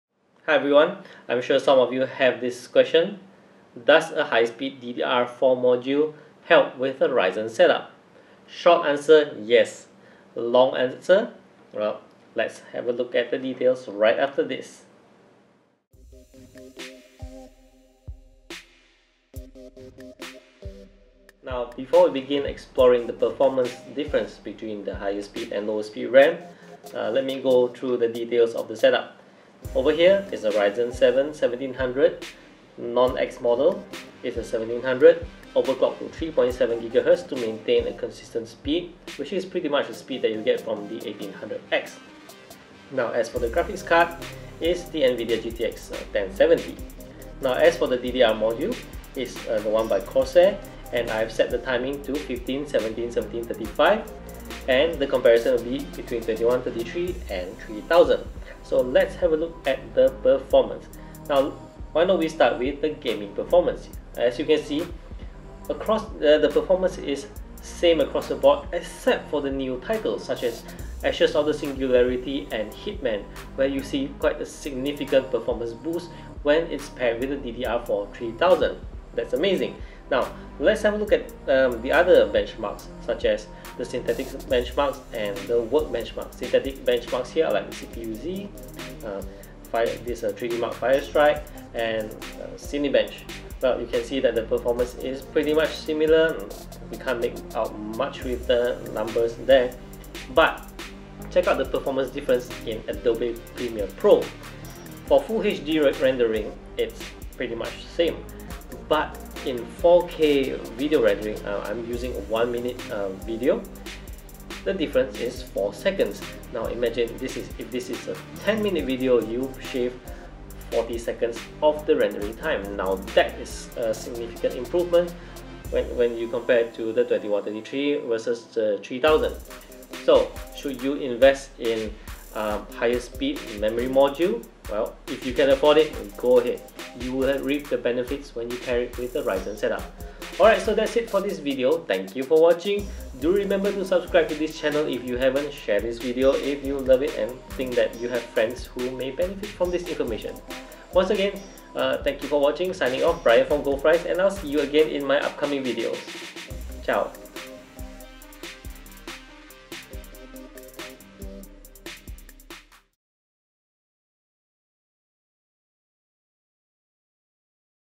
AMD Ryzen 7 Memory Performance - DDR4-2133 vs DDR4-3000 Comparison
Would a low speed RAM suffice or should you be getting high-speed RAM for the Ryzen setup?

Short answer - High speed RAM helps. :) Details in the video.